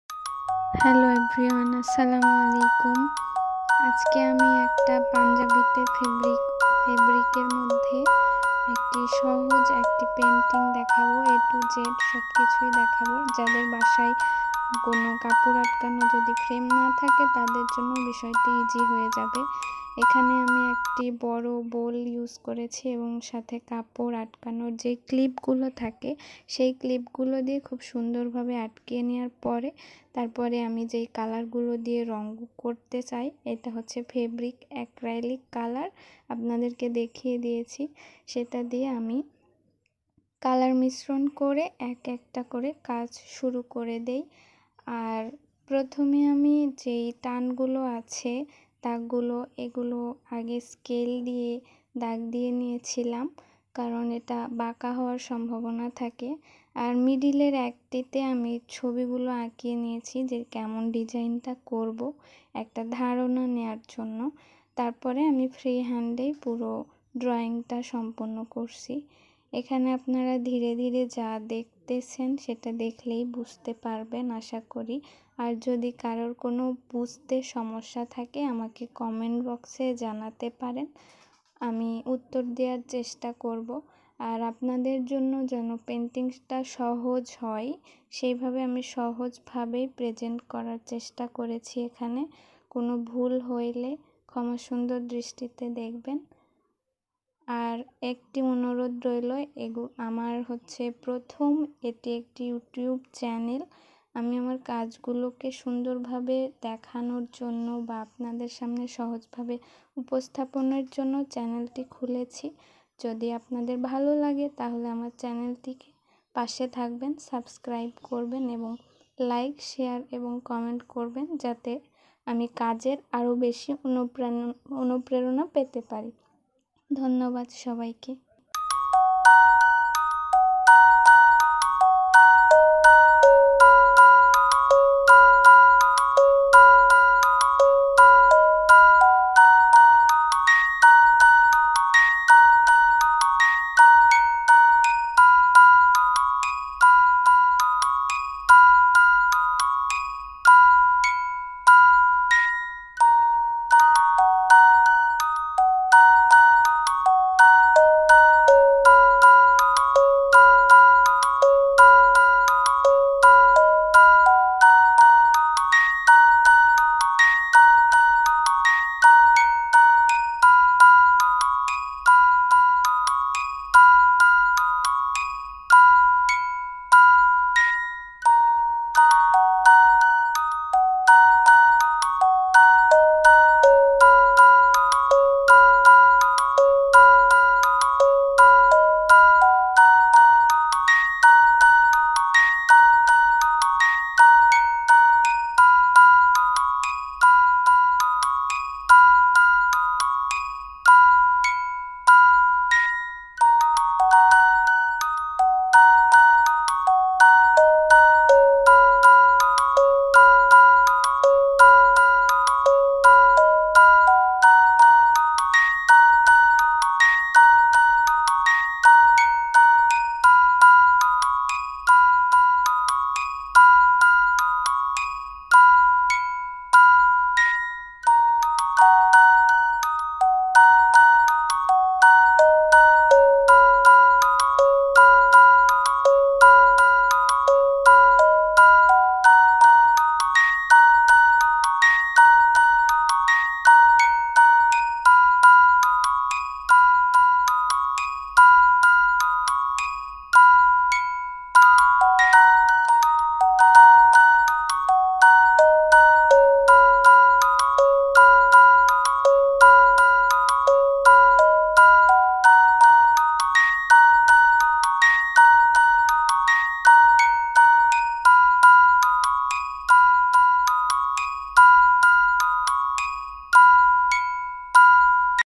punjabi hand painting tutorial/hand painted mandala punjabi drawing ideas/হ্যান্ড পেইন্ট পান্জাবি/
punjabi hand painting tutorial/hand painted mandala punjabi drawing ideas/
হ্যান্ড পেইন্ট পান্জাবি

In this video i show that punjabi hand painting tutorial &hand painted mandala punjabi drawing ideas.
হ্যান্ড পেইন্ট পান্জাবিতে মান্ডালা ডিজাইন টিউটোরিয়াল